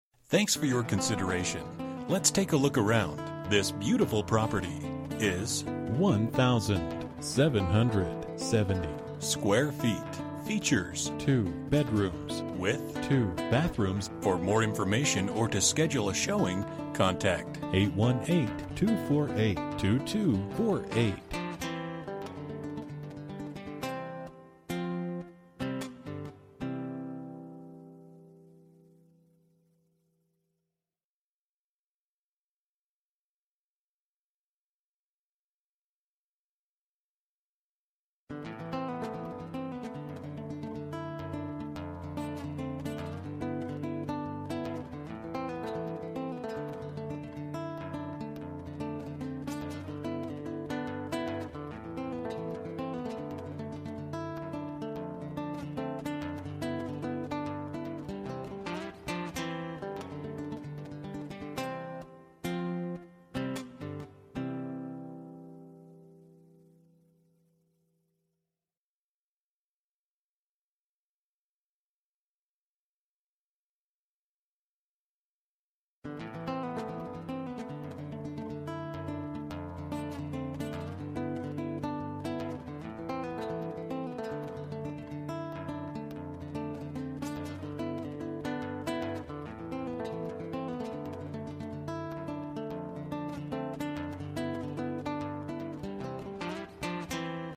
Residential for rent - 395 East 4TH Street 5, Long Beach, CA 90802
This beautiful 2 story loft is located in the heart of the East Village Art District in Downtown Long Beach.  This 1770 sqft 2 bedroom loft offers open floor plan with 22 foot high ceilings, 3 entry access points, hardwood floors, gourmet kitchen with stainless steel appliances, gas stove top, granite counter tops, custom lighting, patio deck with stoup, central heat & AC, master bathroom with side by side sinks, all glass shower door, large tub, in unit washer & dryer, on site security patrol, 2 side by side subterranean parking with caged storage bins.

Bedrooms: 2
Bathrooms: 2 Bathrooms
Square Feet: 1770
Price: $2,900